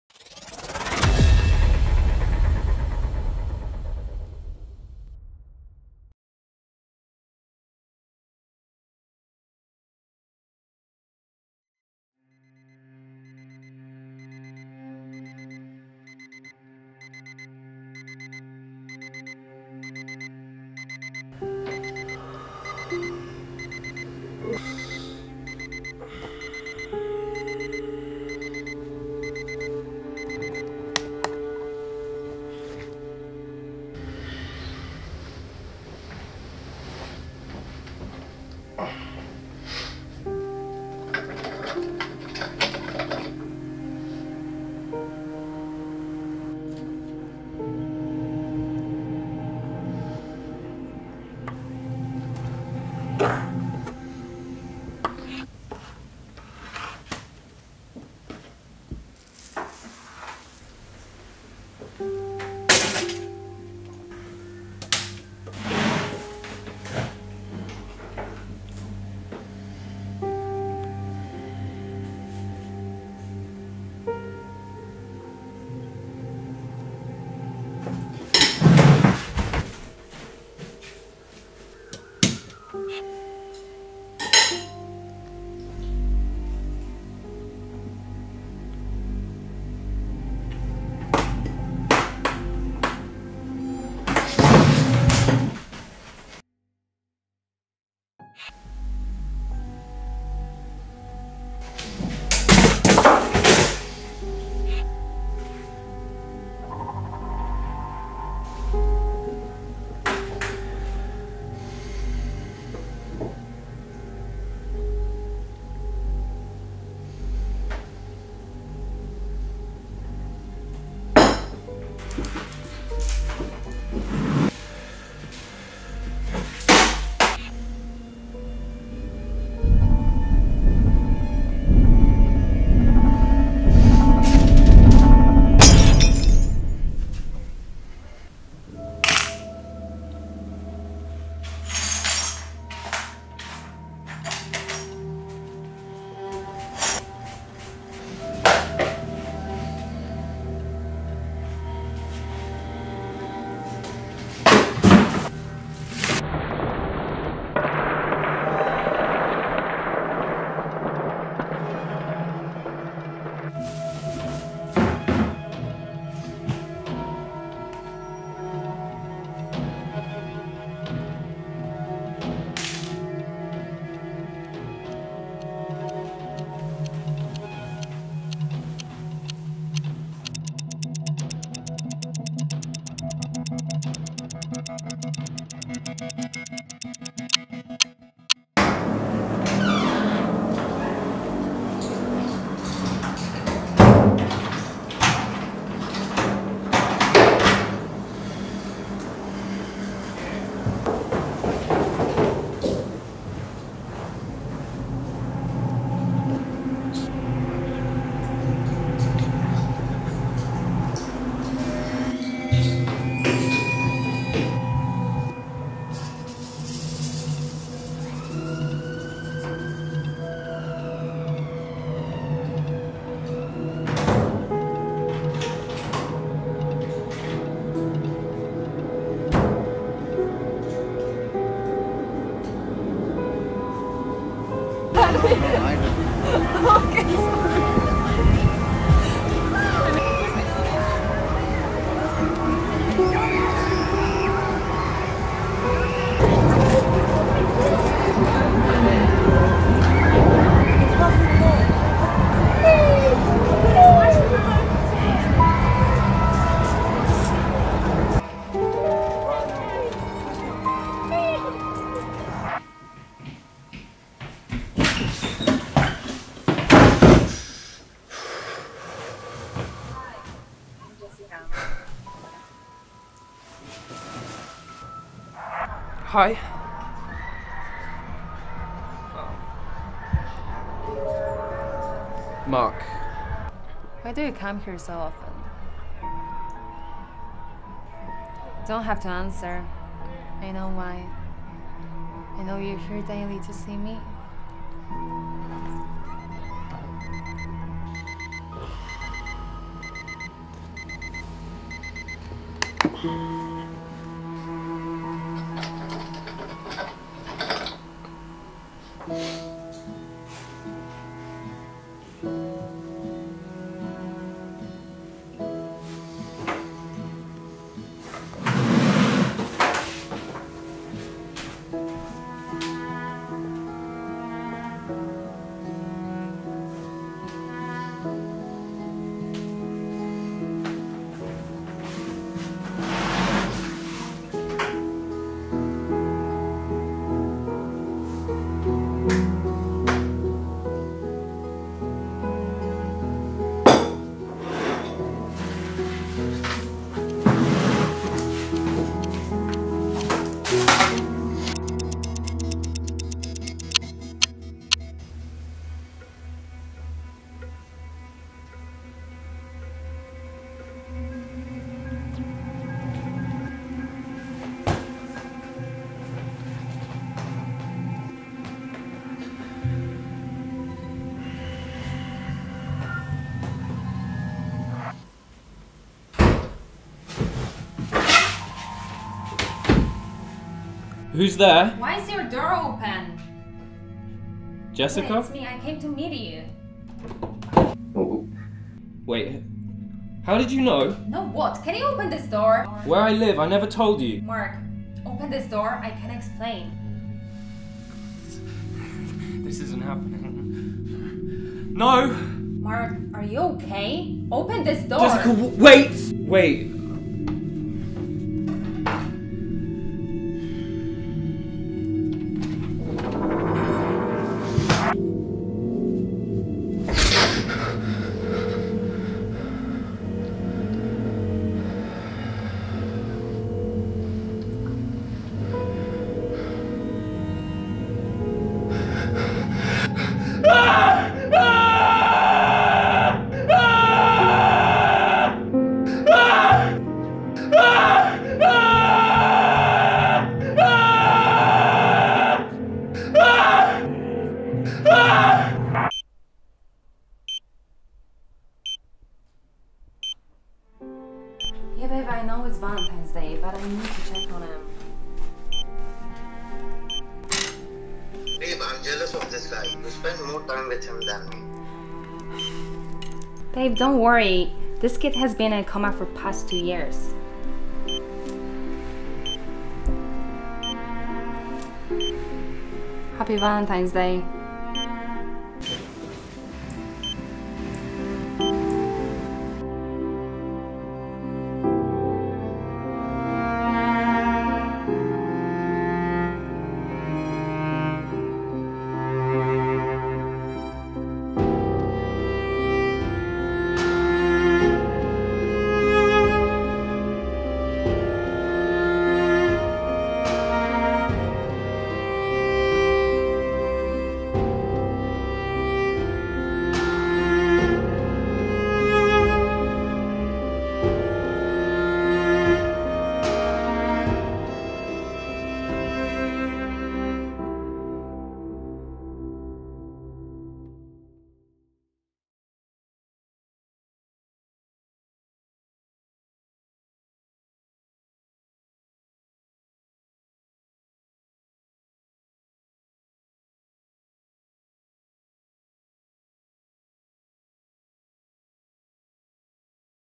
In His Wit
Synopsis: Mark is controlled by his delusions. He can't shake the feeling of being a passenger in his own body; passively watching his own life play out before him as if it were a TV commercial. The hope of romance pushes him beyond fear but, there, so far beyond the comfort of monotony, waits for a dark truth.

This is my first directed project 'In His Wit' hope you like it.